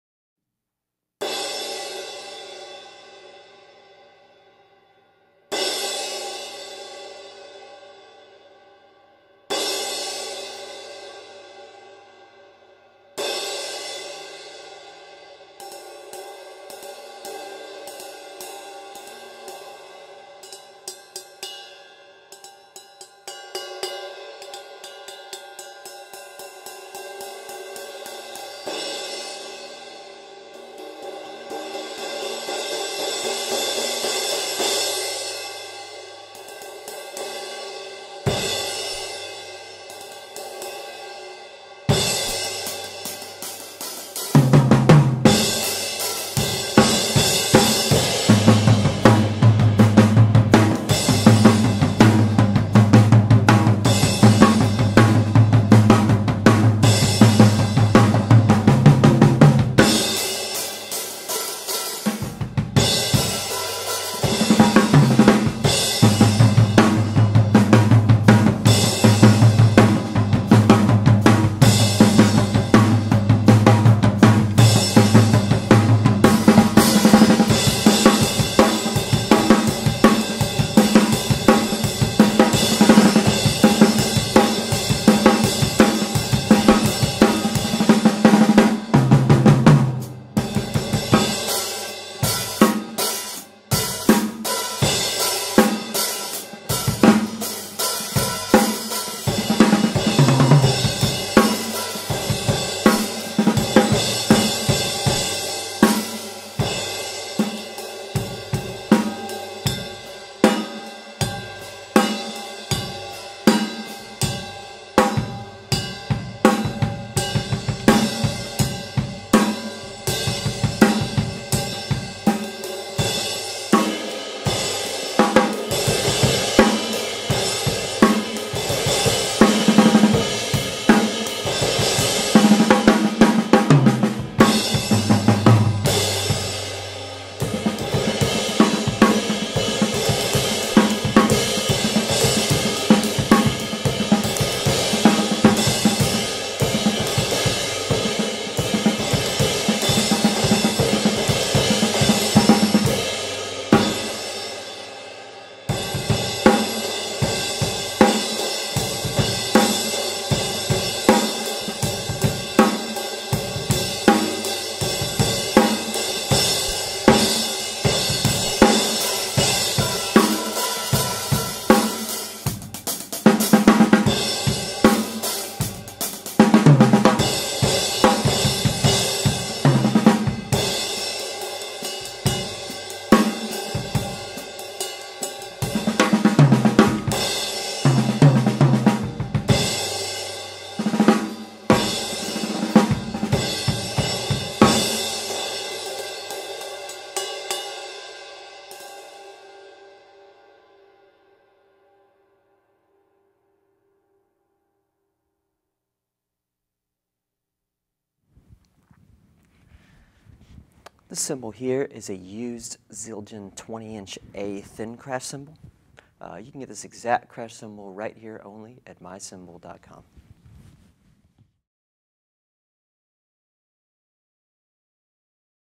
Used Zildjian 20" A Thin Crash Cymbal - 1892g - Played by Shawn Zorn (UA0227-1021516L)
*SOLD* Used Zildjian 20" A Custom Medium Ride Cymbal.

Other gear used in this video:
• Cymbals (From drummers L to R): Zildjian 14" A New Beat Hi Hat Cymbals, Zildjian 22" K Constantinople Medium Thin High Ride Cymbal